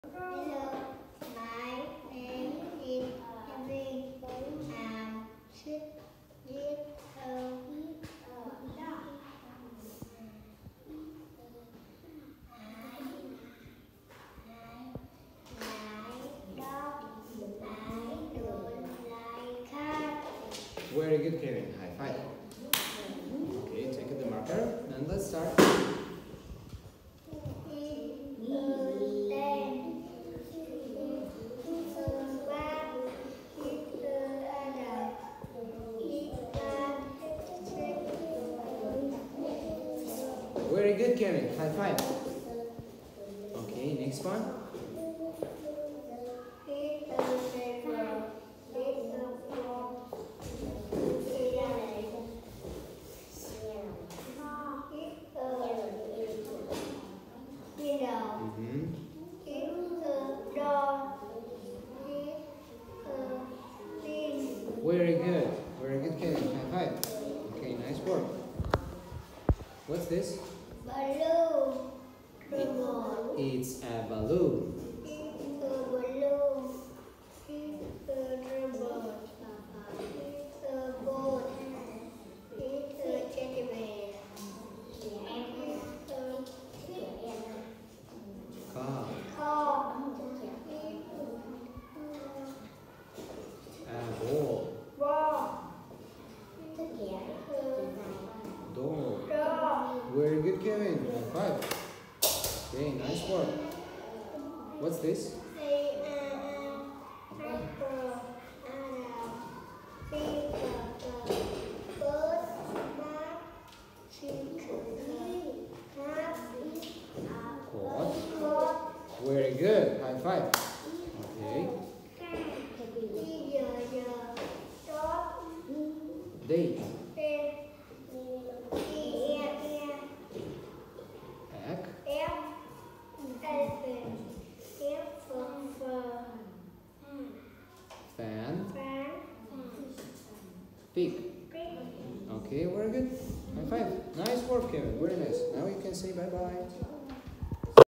ĐỨC PHONG - KELVIN - BEGINNER A5 - ALIBABASCHOOL TAM ĐIỆP
Con nhớ từ vựng và các mẫu câu. Tuy nhiên con cần đọc nhanh hơn chút nữa và chú ý các âm đuôi của một số từ như là ''robot, coat" nữa nhé.